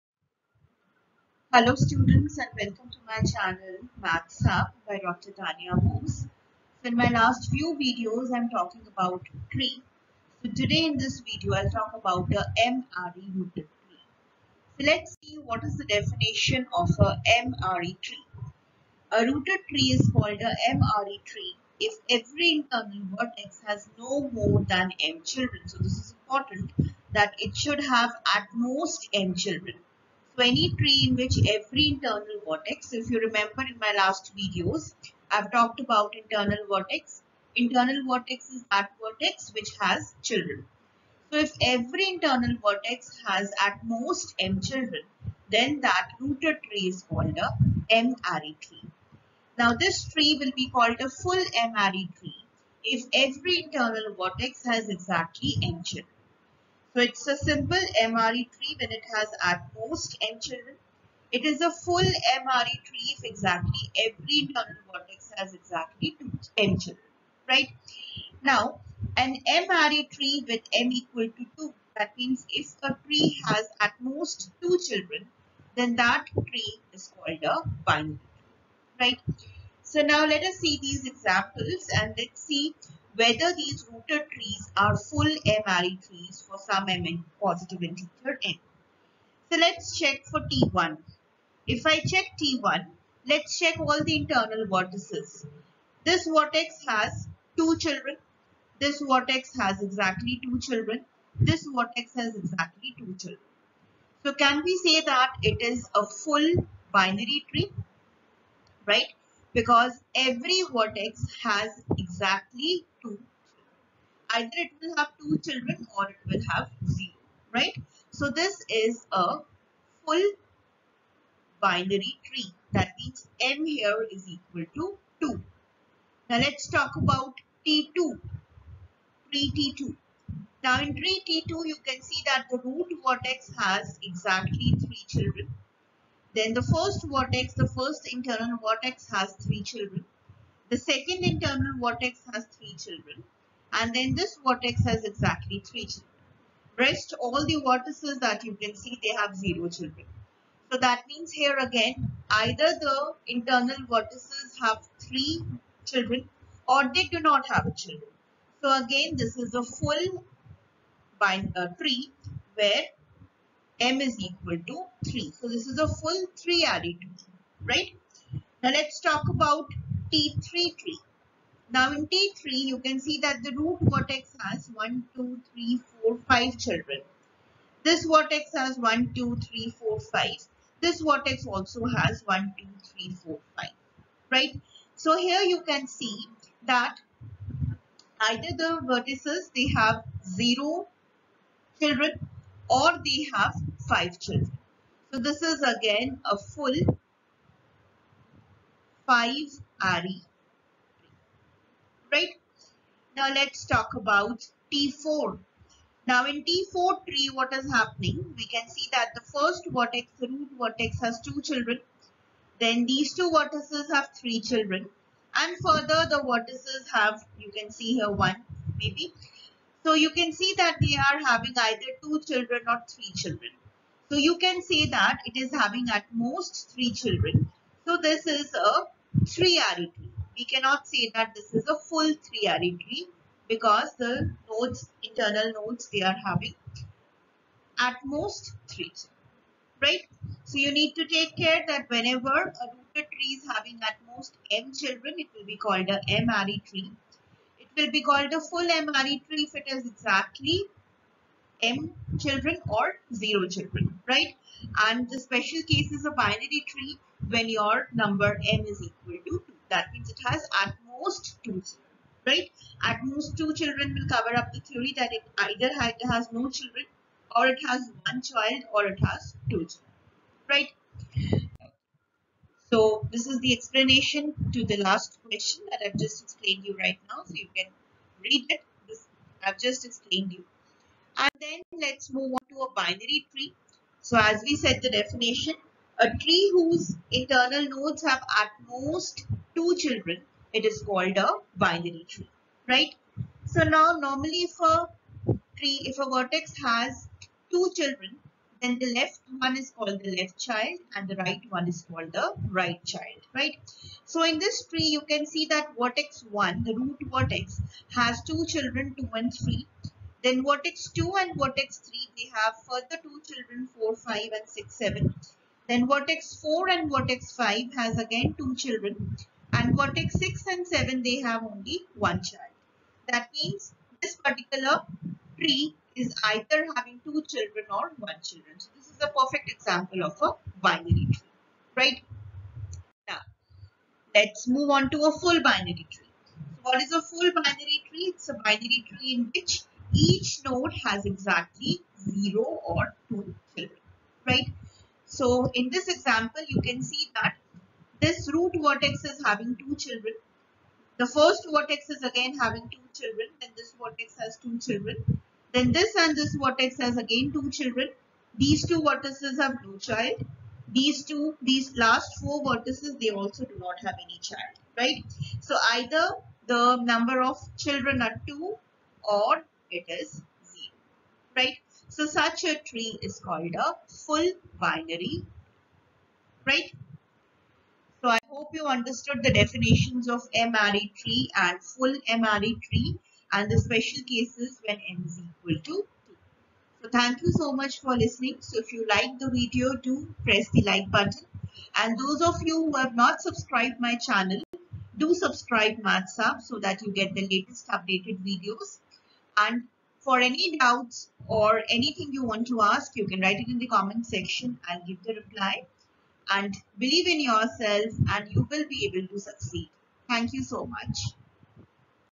m-ary Rooted Tree (Tree Part-4)
This video will tell you about m-ary rooted tree along with suitable examples.

m-aryrootedtree
fullm-aryrootedtree